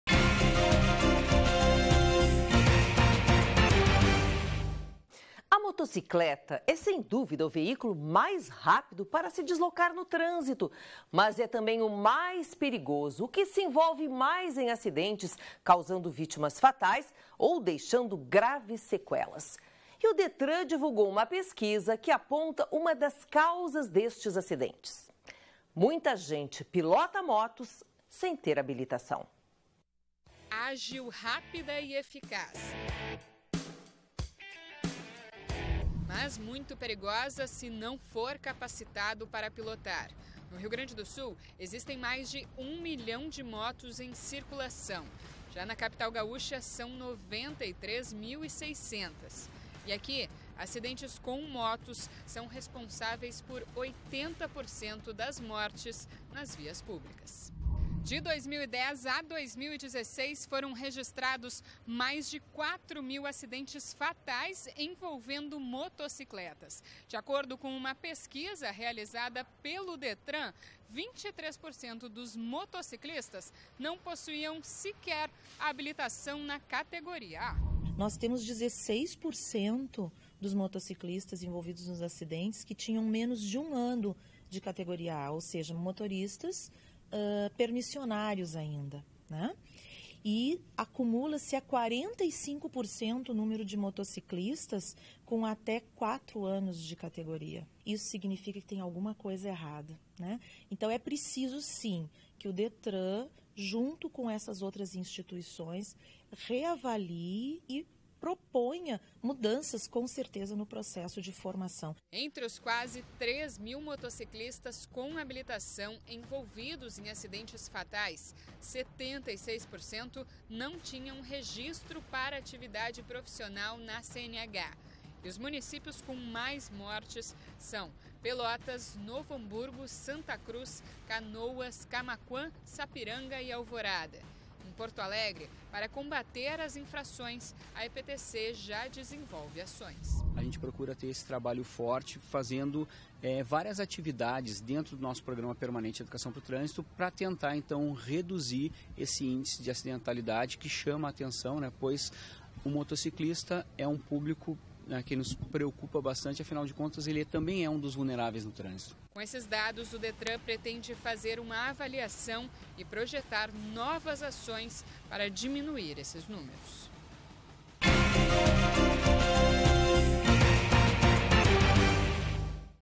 Motociclistas envolvidos em acidentes fatais não são habilitados | Jornal da Pampa | 11/07/2017
23% dos motociclistas envolvidos em acidentes fatais não são habilitados.